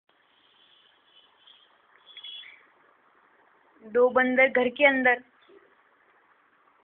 Monkeys at my house..
There were two monkeys which came inside my my house through the window. ! There they are..!!